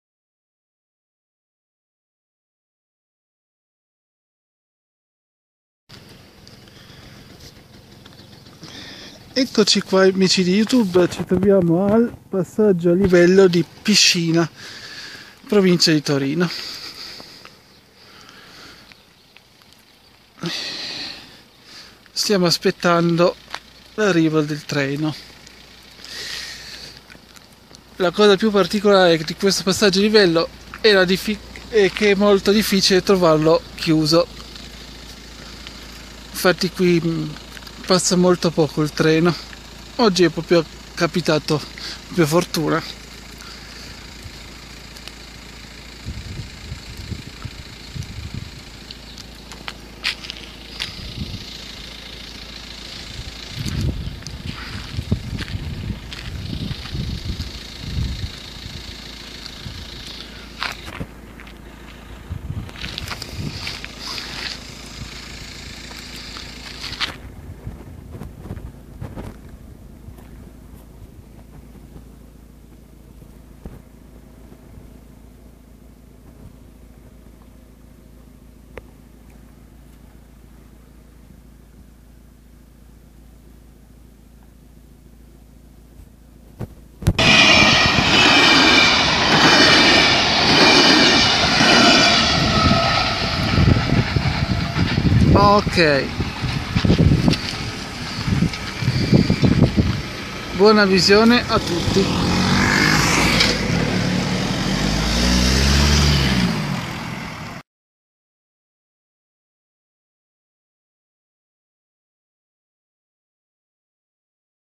Passaggio a livello PISCINA (TO)
Passaggio a livello a Piscina (TO)
questo passaggio a livello è particolare in quanto è raro trovarlo chiuso oggi è stato un caso particolare
video dedicato in particolare modo a Raffaele - Aldo - Daniele

Level crossing in Piscina (TO)
This level crossing is unusual in that it is rare to find it closed today was a special case
video dedicated especially at Raffaele - Aldo - Daniele